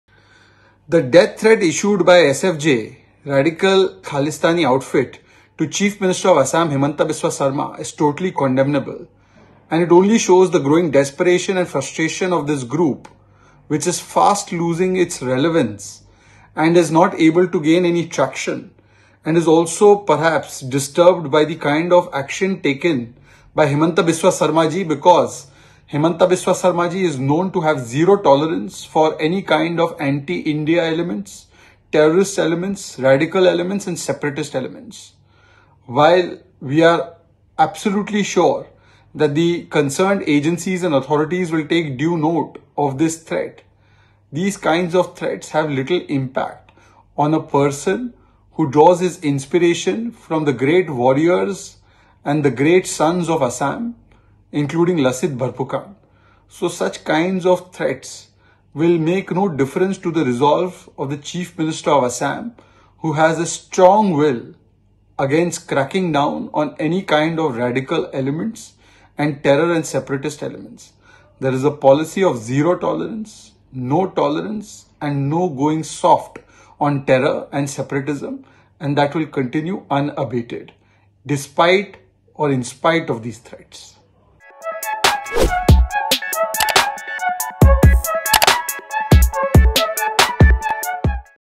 BJP reacts to Assam CM Himanta Biswa Sarma's death threat
🟦🟥 𝗡𝗢𝗥𝗧𝗛𝗘𝗔𝗦𝗧 𝗟𝗜𝗩𝗘 | 𝗘𝗫𝗖𝗟𝗨𝗦𝗜𝗩𝗘
‘Shows its growing frustration’: BJP National Spokesperson Shehzad Poonawalla reacts to Pro Khalistani banned outfit’s death threat to Assam CM. Northeast Live Exclusive